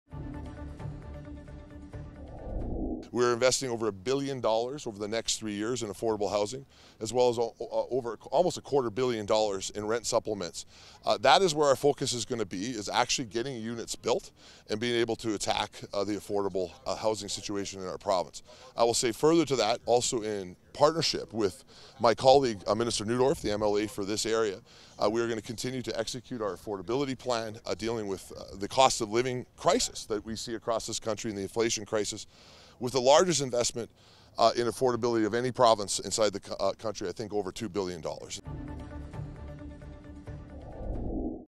Jason Nixon Affordable Housing| Thursday, July 20, 2023 | Angela Stewart | Bridge City News
Aired on: Thursday, July 20, 2023